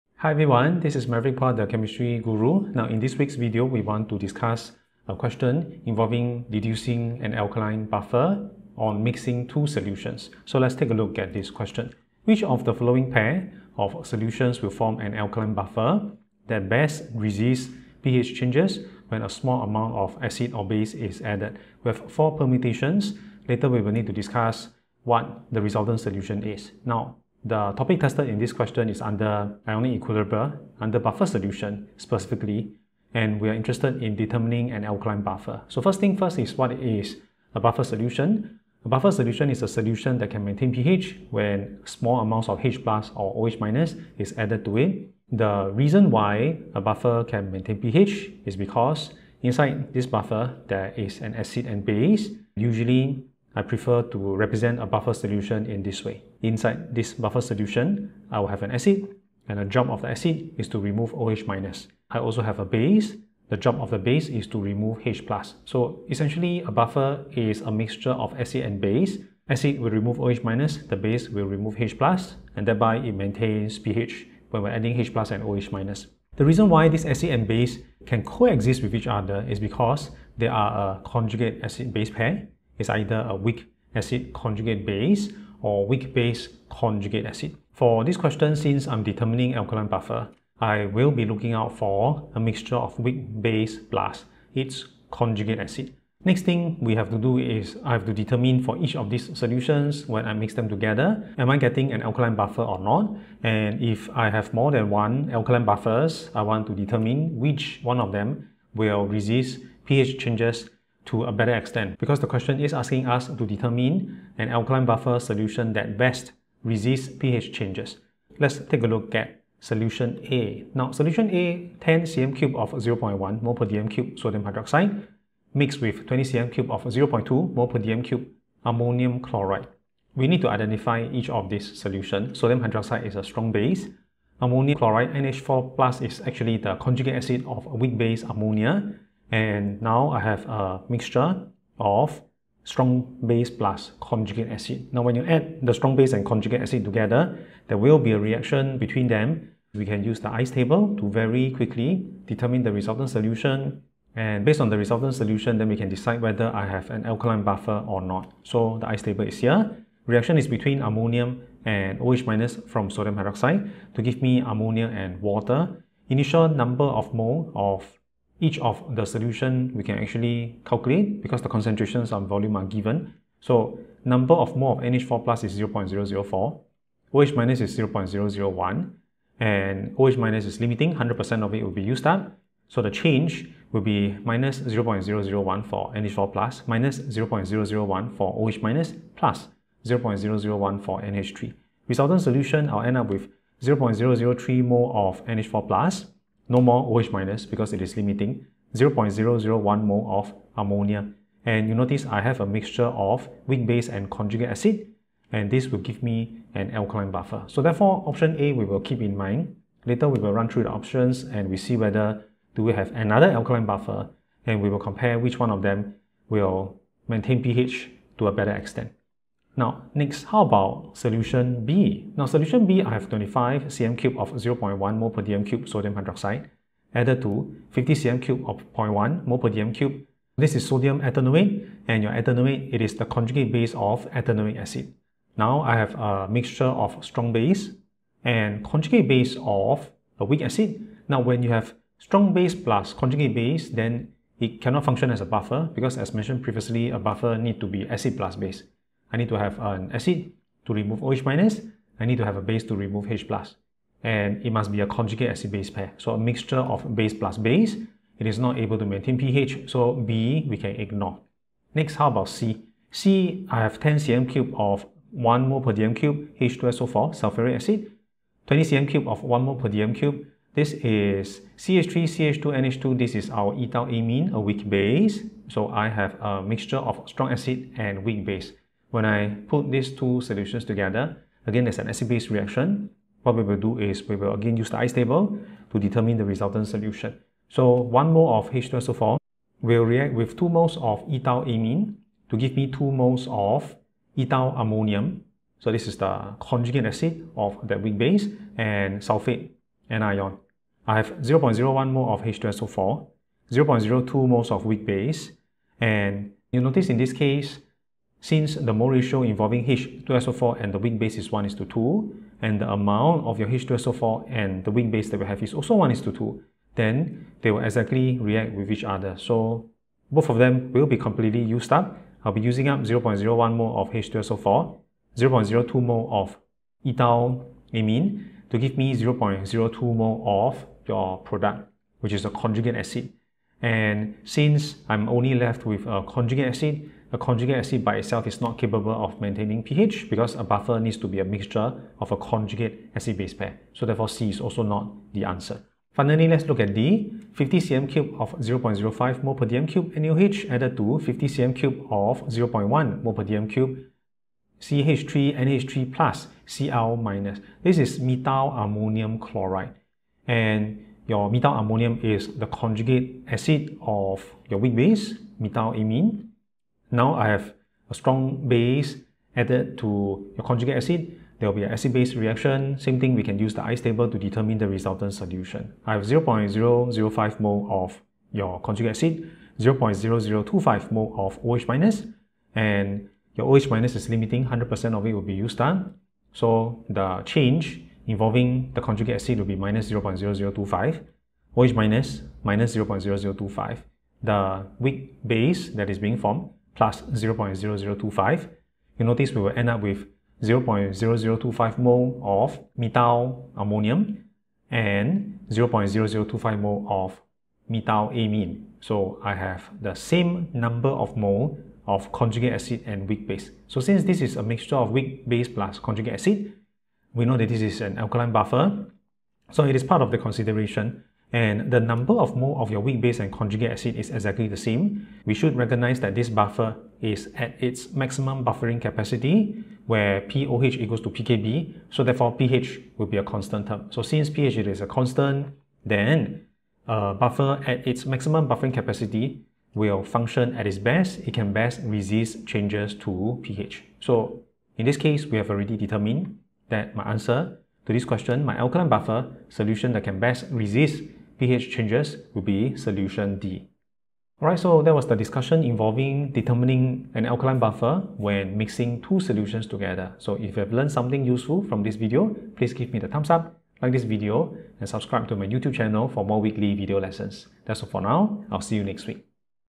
Deduce Alkaline Buffer on Mixing 2 Solutions
A buffer is a solution that maintains pH when small amounts of H+ or OH- is added to it.

It is essentially a mixture of conjugate acid-base pair:
a. acidic buffer = weak acid + conjugate base
b. alkaline buffer = weak base + conjugate acid

The acid will remove OH- added while the base partner will remove H+ added, thereby maintaining pH.

For this question we need to determine the resultant solution when 2 solutions are mixed.

A useful way to determine resultant solution is using the ICE table when there is an acid-base reaction.

A. 10 cm3 of 0.1 mol dm-3 NaOH and 20 cm3 of 0.2 mol dm-3 NH4+Cl-

NaOH is a strong base while NH4+ is conjugate acid of weak base NH3.

Resultant solution is 0.001 mol of weak base NH3 + 0.003 mol conjugate acid NH4+.

This is an alkaline buffer and can be our answer but let's go through the other options first.

B. 25 cm3 of 0.1 mol dm-3 NaOH and 50 cm3 of 0.1 mol dm-3 CH3CO2-Na+

NaOH is a strong base while CH3CO2- is conjugate base of weak acid CH3COOH.

Resultant solution is a mixture of 2 bases which cannot function as a buffer.

C. 10 cm3 of 1 mol dm-3 H2SO4 and 20 cm3 of 1 mol dm-3 CH3CH2NH2

H2SO4 is a strong acid while CH3CH2NH2 is a weak base.

Resultant solution is 0.02 mol of conjugate acid CH3CH2NH3+ which is not a buffer.

D. 50 cm3 of 0.05 mol dm-3 NaOH to 50 cm3 of 0.10 mol dm-3 CH3NH3+Cl-

NaOH is a strong base while CH3NH3+ is conjugate acid of weak base CH3NH2.

Resultant solution is 0.0025 mol of weak base CH3NH2 + 0.0025 mol of conjugate acid CH3NH3+.

We have another alkaline buffer and the moles of weak base and conjugate acid are exactly the same.

This buffer is at its maximum buffering capacity so pH will be a constant and can best resist changes to pH.

Therefore answer to this question is option D.

Topic: Buffer Solution, Physical Chemistry, A Level Chemistry, Singapore